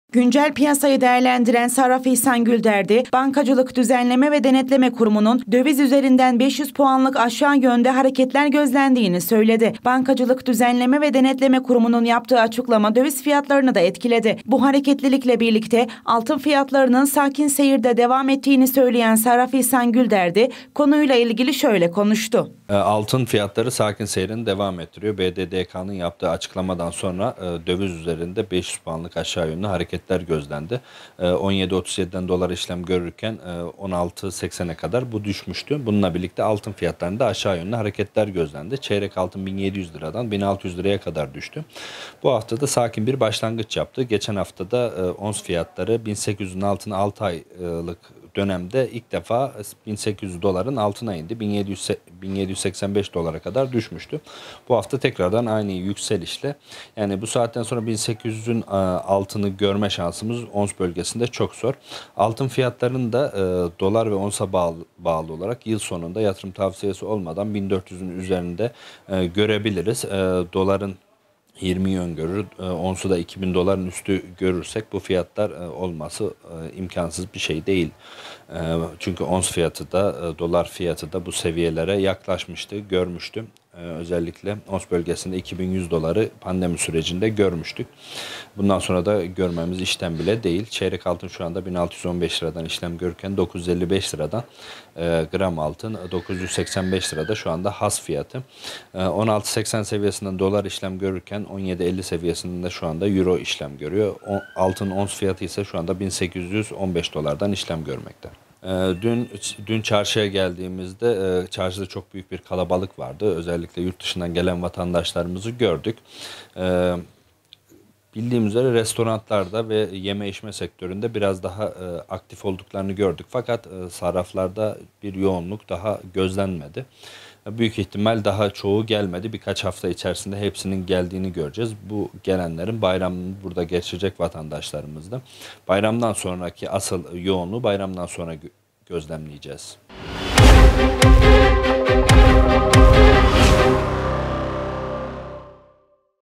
Sarraf İhsan Gülderdi: “Altın fiyatları sakin seyrini koruyor”
KAYSERİ (1HA) - Güncel piyasayı değerlendiren Sarraf İhsan Gülderdi, Bankacılık Düzenleme ve Denetleme Kurumu’nun (BDDK) döviz üzerinden 500 puanlık aşağı yönde hareketler gözlendiğini söyledi.

Bankacılık Düzenleme ve Denetleme Kurumu’nun (BDDK) yaptığı açıklama döviz fiyatlarını da etkiledi. Bu hareketlilikle birlikte altın fiyatlarının sakin seyirde devam ettiğini söyleyen Sarraf İhsan Gülderdi, “BDDK’nın yaptığı açıklamadan sonra döviz üzerinde 500 puanlık aşağı yönde hareketler gözlendi. 17,37’den dolar işlem görürken 16,80’e kadar bu düşmüştü. Bununla birlikte altın fiyatlarında da aşağı yönlü hareketler gözlendi. Çeyrek altın 1700 liradan 1600 liraya kadar düştü. Bu haftada sakin bir başlangıç yaptık. Geçen haftada ONS fiyatları 6 aylık dönemde ilk defa bin 800 doların altına indi. ONS fiyatı bin 785 Dolara kadar düşmüştü bu hafta tekrardan aynı yükselişe geçti. Bu saatten sonra ONS fiyatının bin 800 doların altında görme şansımız çok zor. Altın fiyatları da Dolar ve ONS fiyatlarına bağlı olarak yılsonunda bin 400’ün üzerinde görebiliriz” şeklinde konuştu.
Şuanda dükkanlarda çok fazla yoğunluk olmadığını dile getiren Gülderdi, “Dün çarşıda çok büyük bir kalabalık vardı. Özellikle yurtdışından gelen vatandaşlarımız gördük. Yurtdışından gelen vatandaşların genellikle restoranlarda, yeme içme sektörlerinde biraz daha aktif olduklarını gördük. Sarraflarda şuan için bir yoğunluk gözlenmedi. Büyük ihtimalle birçoğu daha gelmedi. Birkaç hafta içerisinde hepsinin geldiğini göreceğiz. Asıl yoğunluğu bayramdan sonra gözlemleyeceğiz” diye konuştu.

Güncel altın ve döviz fiyatları ise şu şekilde: Çeyrek altın: bin 615 lira, 22 ayar gram altın: 955 lira, Has altın: 985, ONS: bin 815 lira, Euro: 17,50 lira, Dolar: 16,80 lira.